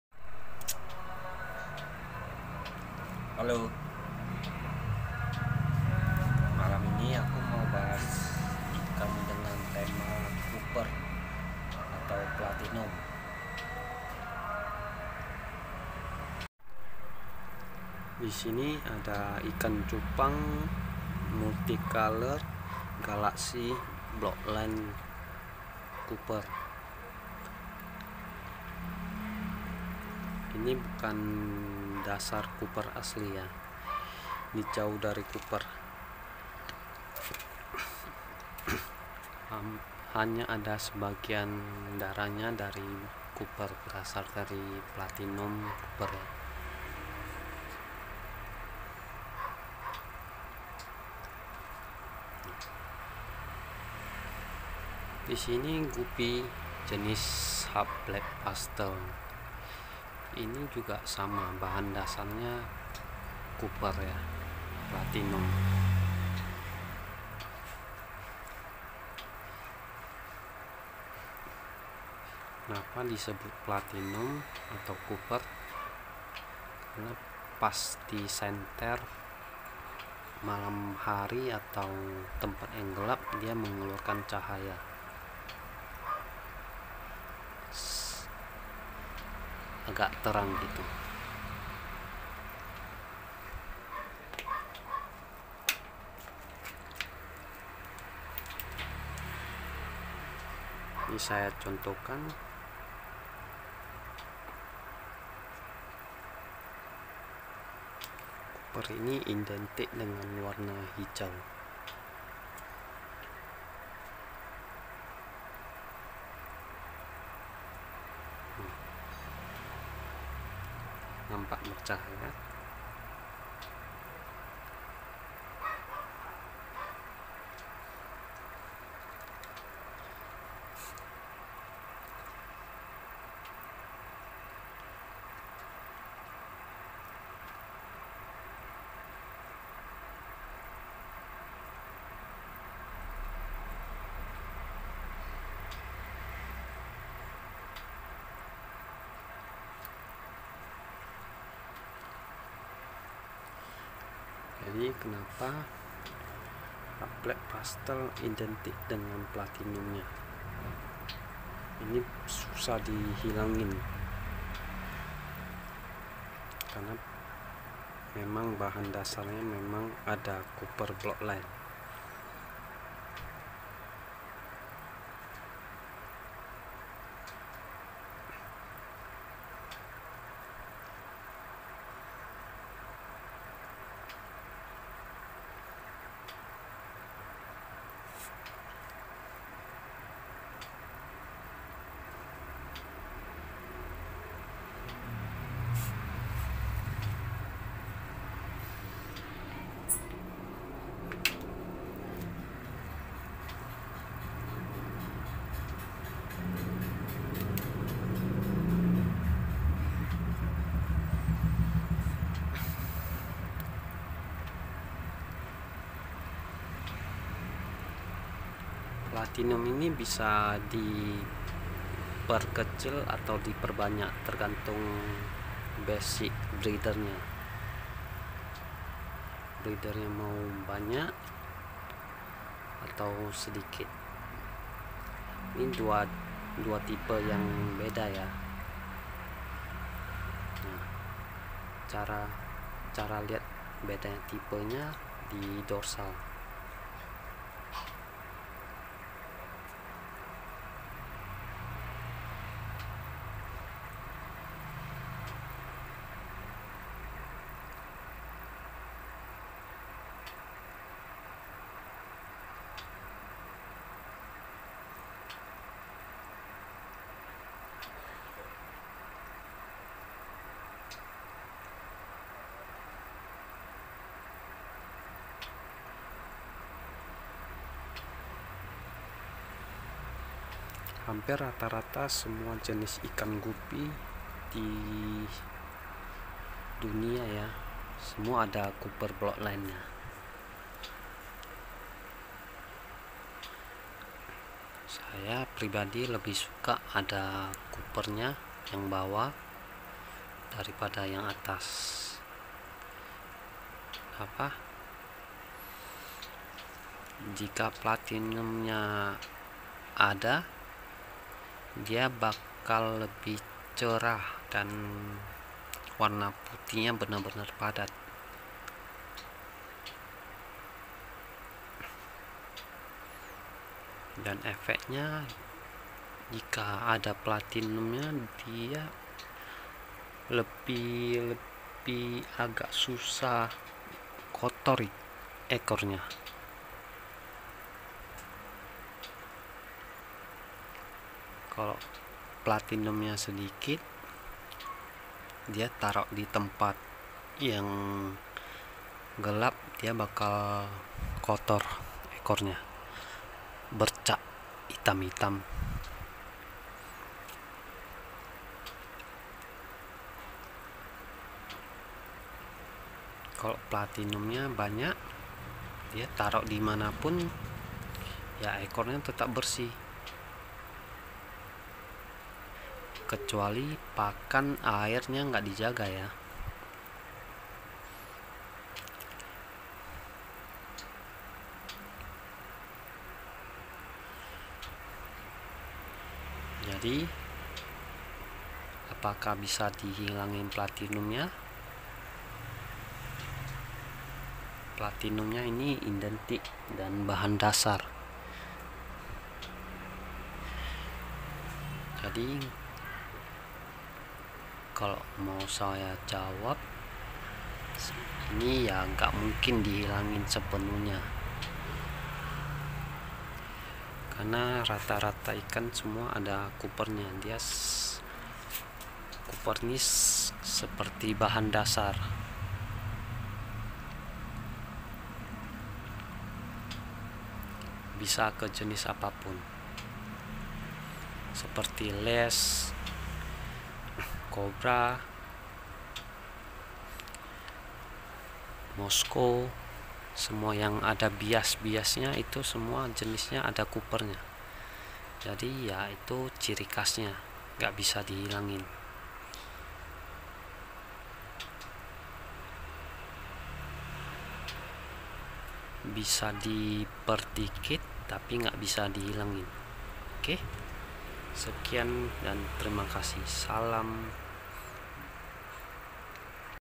Platinum hb pastel apa itu?dan asal usul nya dari mana
ikan cupang dan ikan guppy banyak kesamaan dan banyak pelajaran yang bisa di pelajarin,salah satu tentang bloodline copper